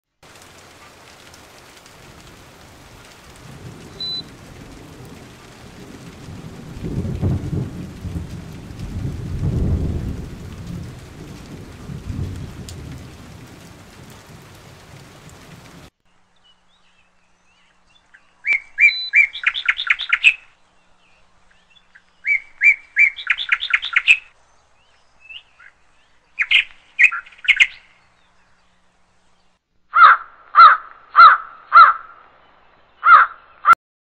Quick Time Lapse
Here is a quick time lapse short I created from my back yard security camera